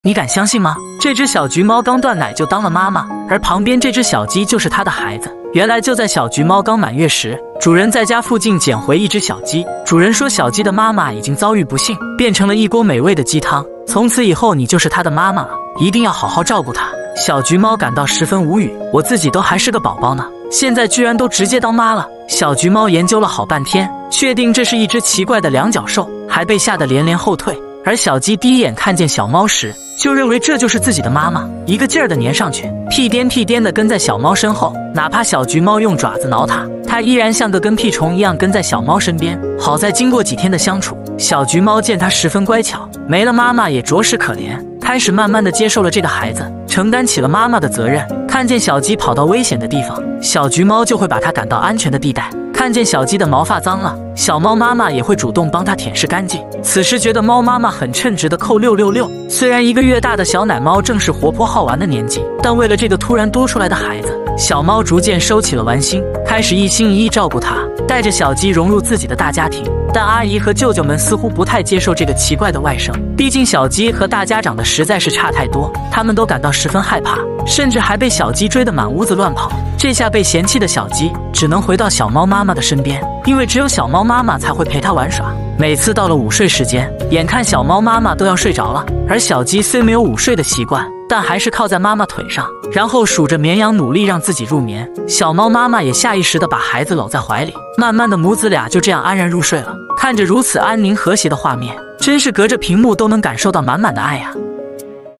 생후 1개월 된 우유고양이가 강제로 엄마가 되어 닭을 키우기 시작했어요 마음이 너무 귀여워서 봤어요 작은 우유고양이의 성장일기 폭발에 귀여움 💥 고양이 #
생후 1개월 된 우유고양이가 강제로 엄마가 되어 닭을 키우기 시작했어요 마음이 너무 귀여워서 봤어요 작은 우유고양이의 성장일기 폭발에 귀여움 💥 고양이 # 아기 우유 고양이의 성장 일기, 너무 귀여워 폭발